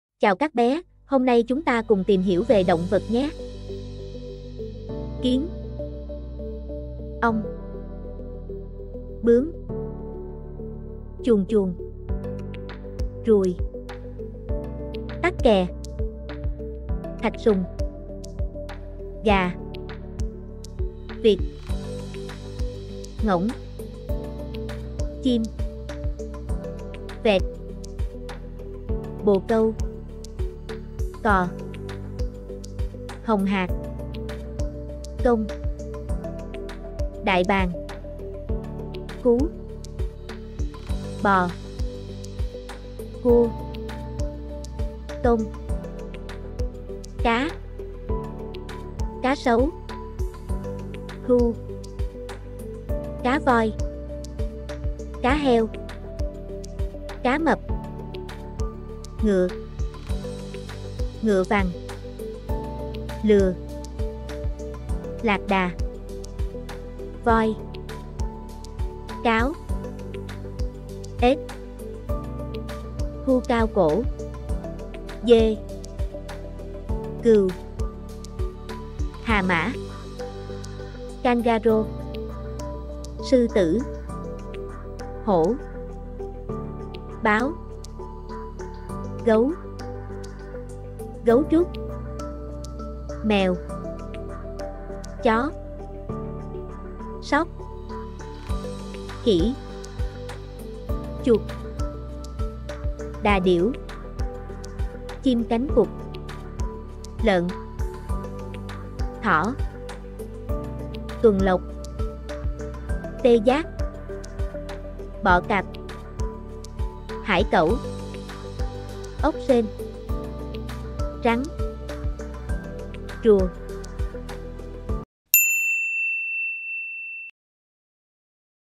Bài 4: Dạy bé nhận biết ĐỘNG VẬT | Glenn Doman | Giáo dục sớm
🔍Hôm nay chúng ta sẽ cùng các bé tìm hiểu về 60 loài Động vật bên dưới nhé: